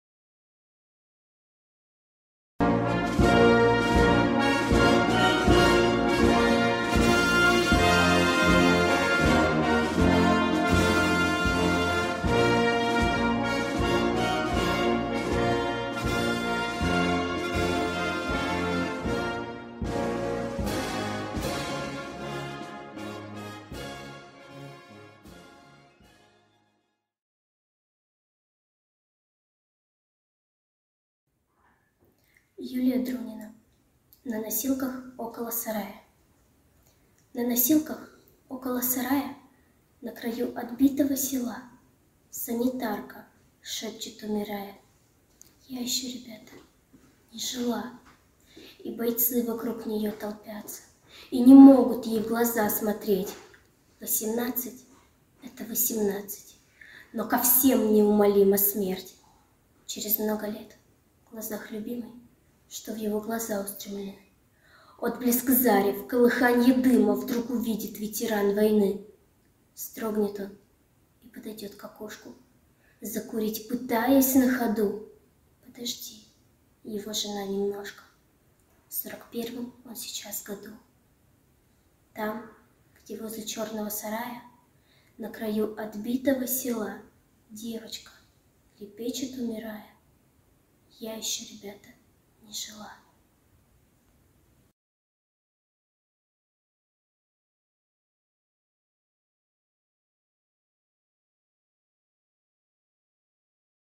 Юлия Друнина, "На носилках около сарая…", читает Вероника Тараканова, 14 лет, г. Каргополь
Юлия Друнина, "На носилках около сарая…", читает Вероника Тараканова, 14 лет, г. Каргополь, Архангельская область.
МБУК "Каргопольская ЦБС".

Юлия Владимировна Друнина (10 мая 1924 — 21 ноября 1991) — советский поэт. Член Союза писателей СССР. Секретарь Союза писателей СССР и Союза писателей РСФСР. Родилась 10 мая 1924 в Москве в учительской семье.
В 1941 добровольцем ушла на фронт (сначала в авиаполк на Дальнем Востоке, затем санинструктором на 2-м Белорусском и 3-м Прибалтийском фронтах). В 1943 году была тяжело ранена. После излечения вернулась на фронт, попала в 1038-й самоходный артиллерийский полк 3-го Прибалтийского фронта. Воевала в Псковской области, затем в Прибалтике. В одном из боёв была контужена и 21 ноября 1944 года признана негодной к несению военной службы.
Стихи писала с детства, публиковала с 1945 (подборка в журнале «Знамя»); первый сборник – В солдатской шинели (1948).